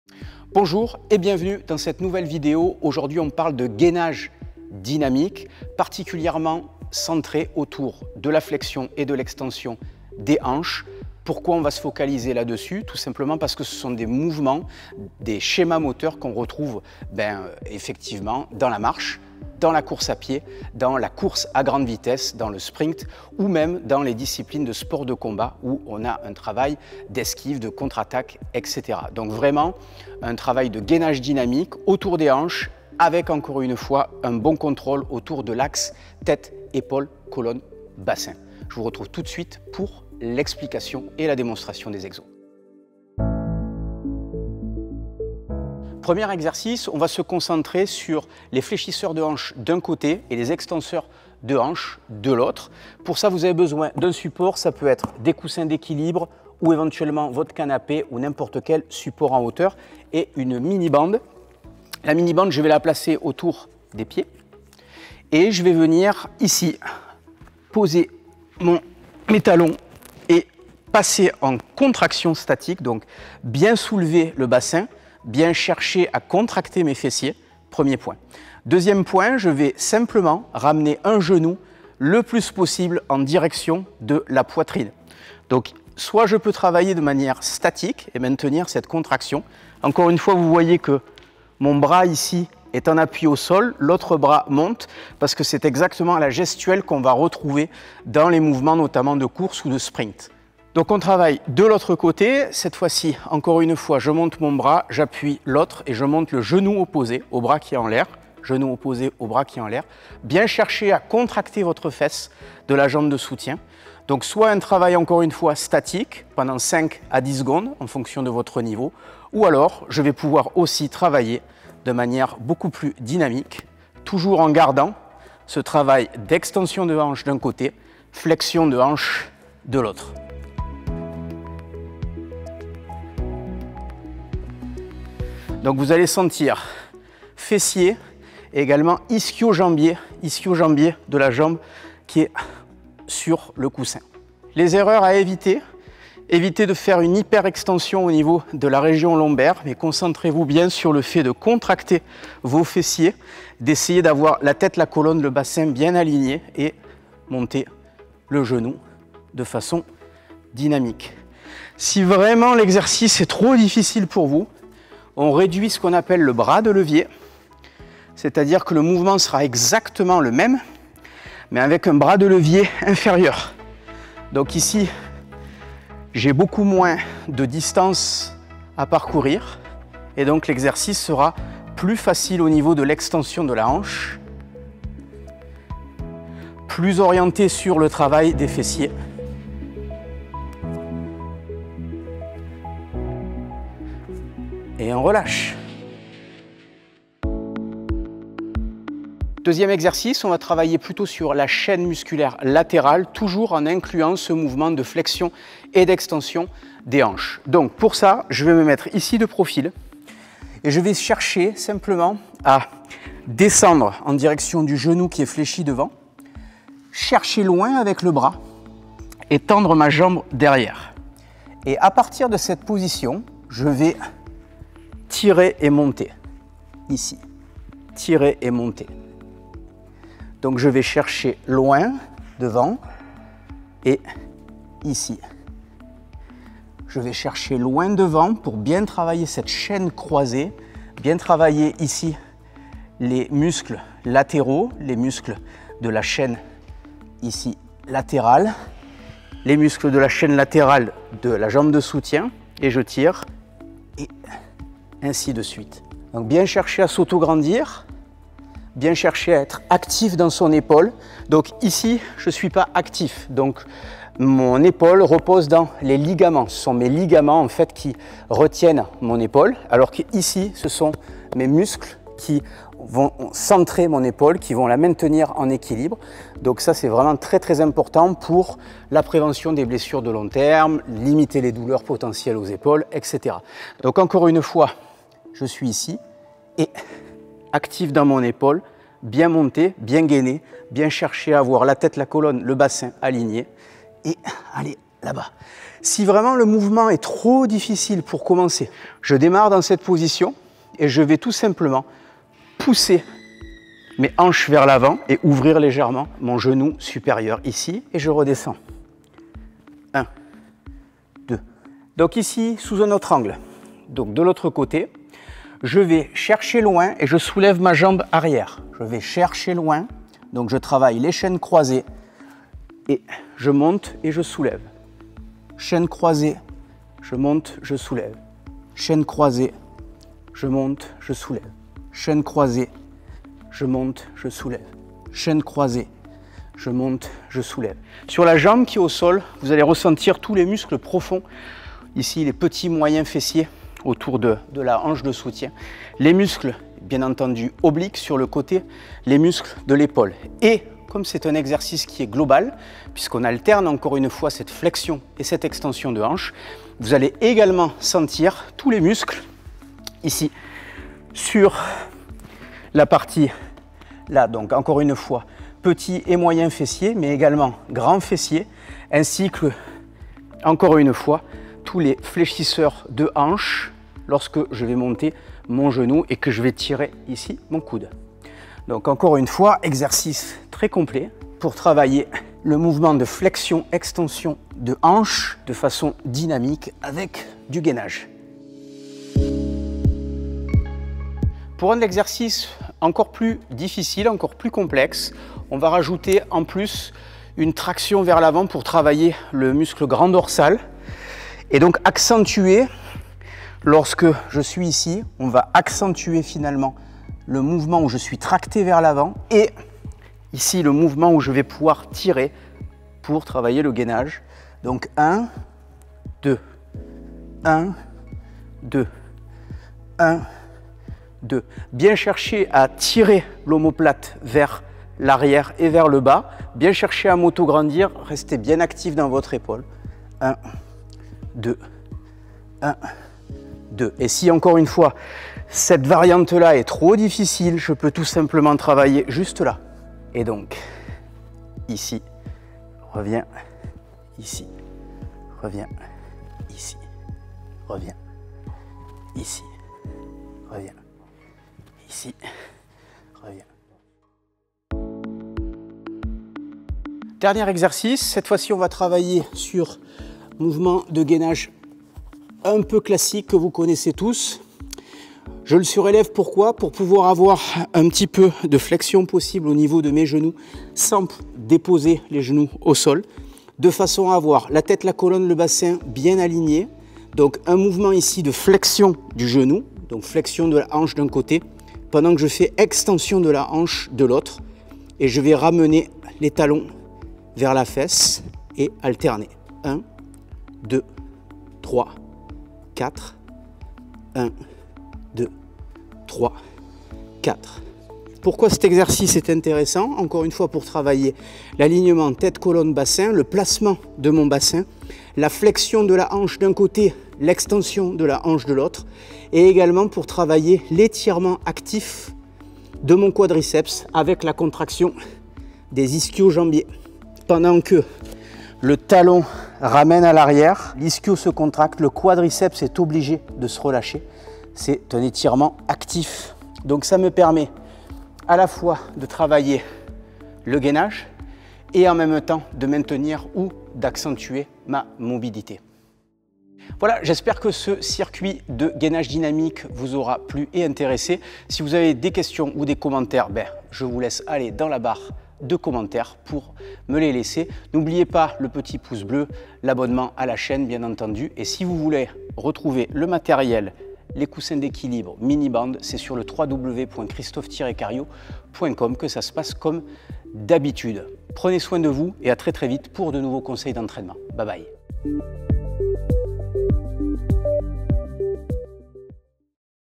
CIRCUIT GAINAGE 2.0
⇩ ARTICLE COMPLEMENTAIRE ET BOUTIQUE ⇩

Cette vidéo parle de quoi ?
Un circuit complet pour travailler le gainage et la mobilité et améliorer les performances en course à pied, sprint pour toute discipline et sport de combat.
Christophe Carrio